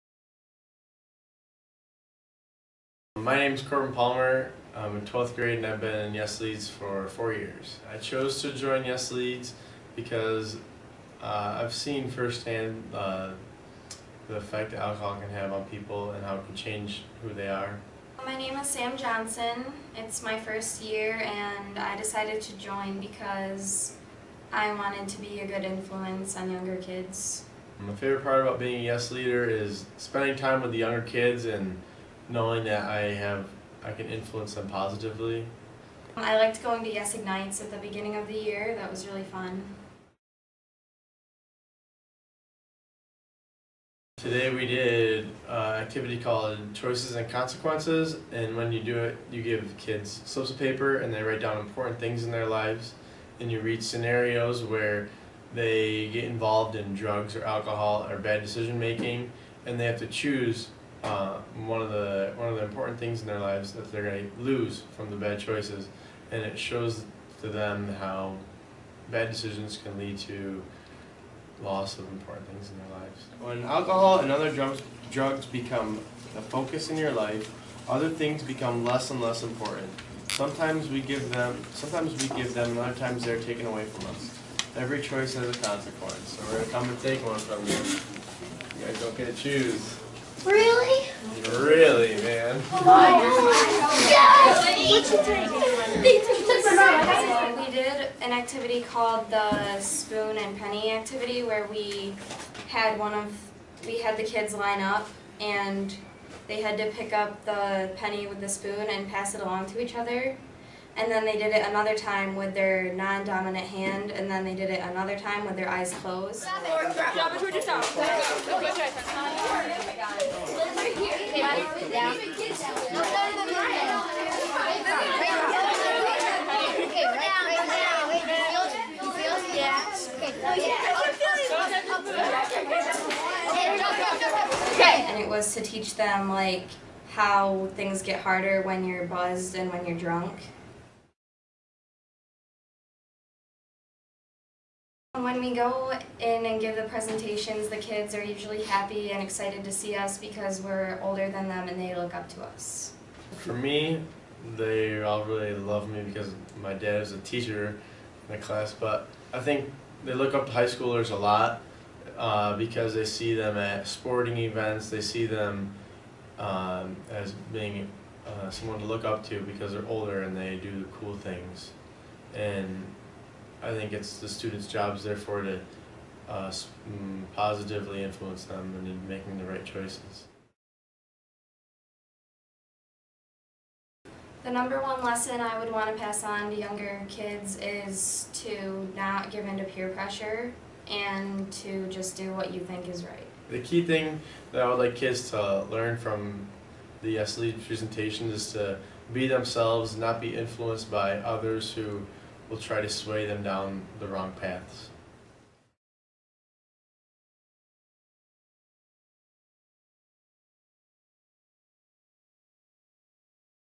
Bainbridge-Guilford High School YES! LEADS Profile
Two YES! LEADers from Bainbridge-Guilford High School talk about their experiences in YES! LEADS and offer their thoughts on their presentations at Greenlawn Elementary School.